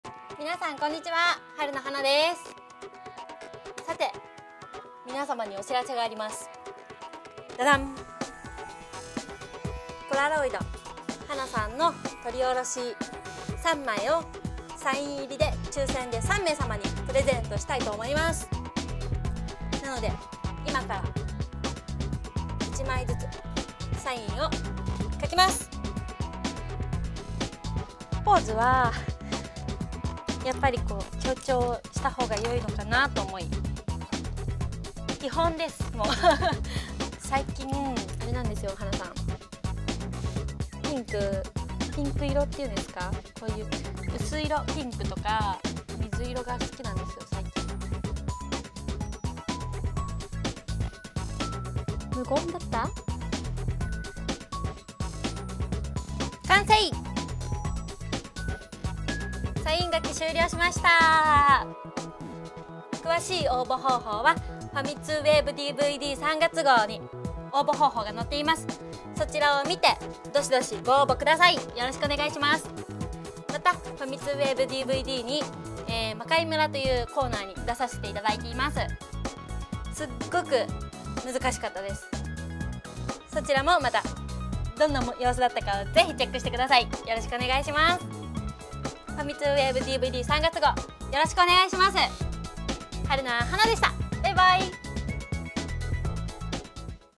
16_FWD_Blog 春菜はな
【ebten】
●ファミ通WaveDVD3月号『魔界むらっ！』に出演する春菜はなさんの映像コメントです。サイン入り生ポラ写真を読者3名にプレゼント！　詳しくは、発売中のファミ通WaveDVD3月号をご覧ください。